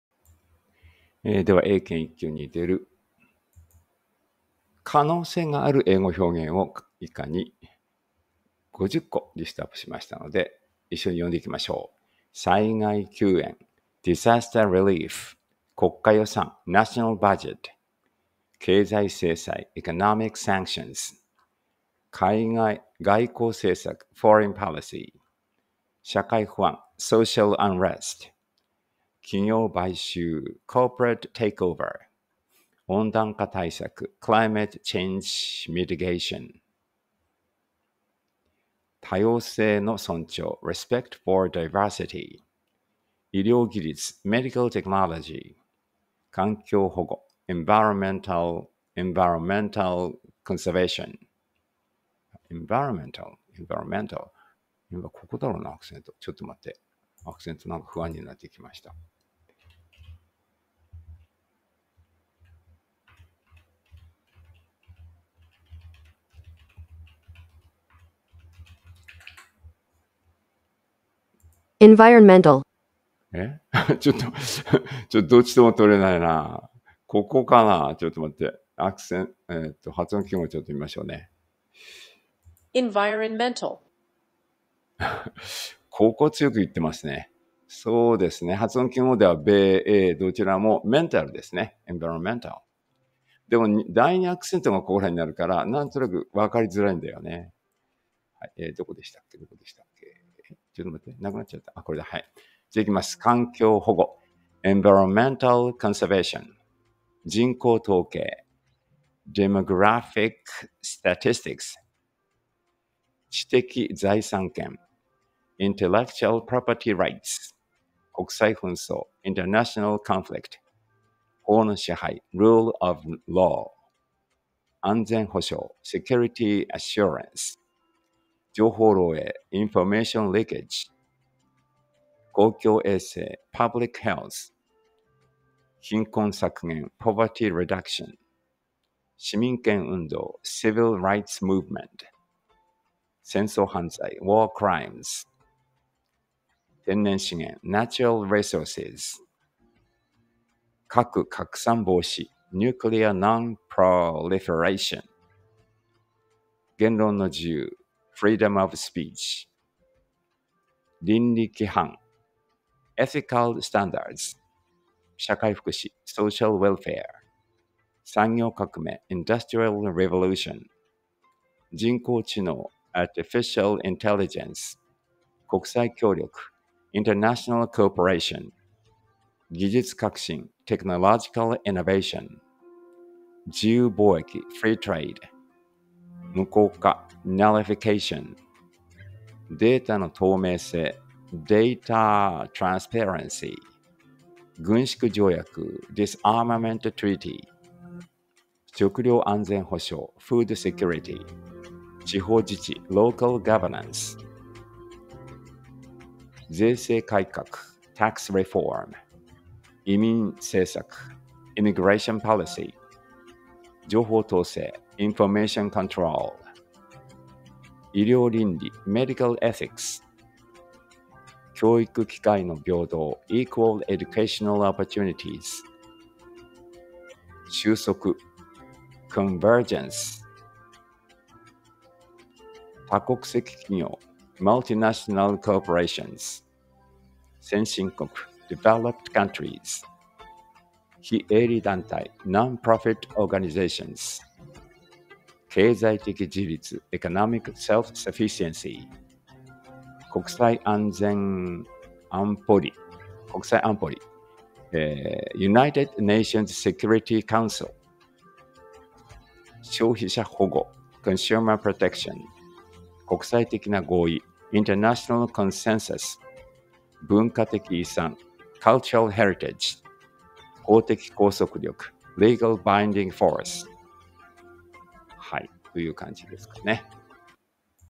えいご おんどく 338 英検1級にでる表現
英検1級に出る可能性がある英語表現を、以下に日本語訳とともに50個リストアップします。

1. **災害救援**
   Disaster relief

2. **国家予算**
   National budget

3. **経済制裁**
   Economic sanctions

4. **外交政策**
   Foreign policy

5. **社会不安**
   Social unrest

6. **企業買収**
   Corporate takeover

7. **温暖化対策**
   Climate change mitigation

8. **多様性の尊重**
   Respect for diversity

9. **医療技術**
   Medical technology

10. **環境保護**
    Environmental conservation

11. **人口統計**
    Demographic statistics

12. **知的財産権**

13. **国際紛争**
    International conflict

14. **法の支配**
    Rule of law

15. **安全保障**
    Security assurance

16. **情報漏洩**
    Information leakage

17. **公衆衛生**
    Public health

18. **貧困削減**
    Poverty reduction

19. **市民権運動**
    Civil rights movement

20. **戦争犯罪**
    War crimes

21. **天然資源**
    Natural resources

22. **核拡散防止**
    Nuclear non-proliferation

23. **言論の自由**

24. **倫理規範**
    Ethical standards

25. **社会福祉**
    Social welfare

26. **産業革命**
    Industrial revolution

27. **人工知能**
    Artificial intelligence

28. **国際協力**
    International cooperation

29. **技術革新**
    Technological innovation

30. **自由貿易**
    Free trade

31. **無効化**
    Nullification

32. **データの透明性**
    Data transparency

33. **軍縮条約**
    Disarmament treaty

34. **食糧安全保障**
    Food security

35. **地方自治**
    Local governance

36. **税制改革**
    Tax reform

37. **移民政策**
    Immigration policy

38. **情報統制**
    Information control

39. **医療倫理**
    Medical ethics

40. **教育機会の平等**
    Equal educational opportunities

41. **収束**
    Convergence

42. **多国籍企業**
    Multinational corporations

43. **先進国**
    Developed countries

44. **非営利団体**
    Non-profit organizations

45. **経済的自立**
    Economic self-sufficiency

46. **国連安保理**
    United Nations Security Council

47. **消費者保護**
    Consumer protection

48. **国際的な合意**
    International consensus

49. **文化的遺産**
    Cultural heritage

50. **法的拘束力**
    Legal binding force

これらの表現は、英検1級に出題される可能性があり、幅広いトピックをカバーしています。